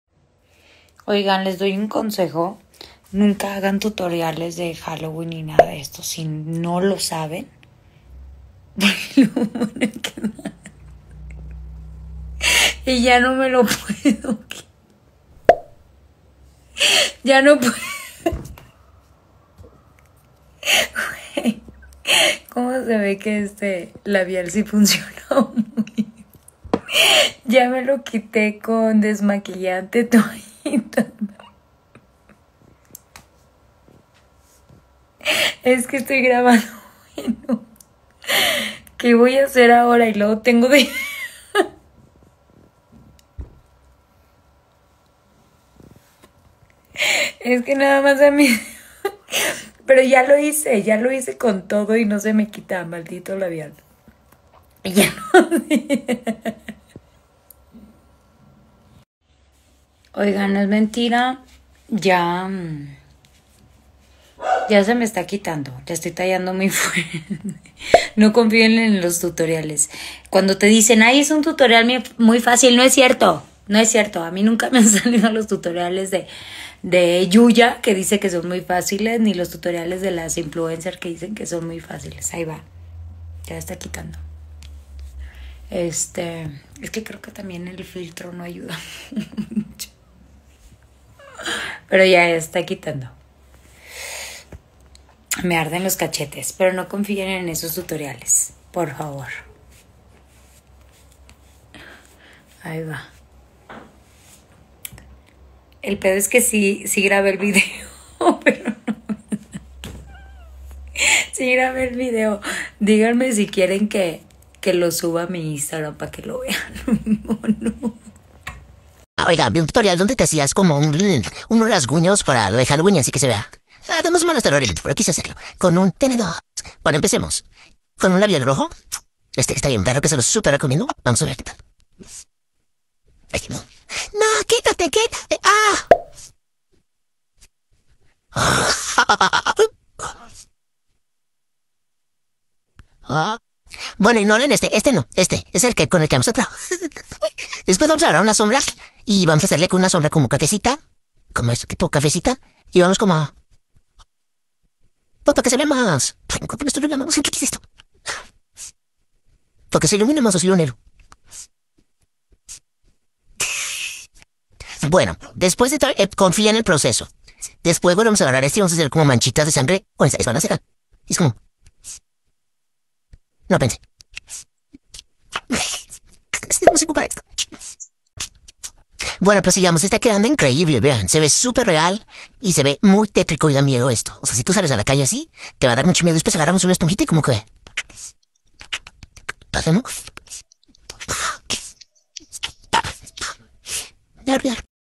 Así quedó Gaby después de seguir un tutorial de maquillaje para halloween.
. 11 octubre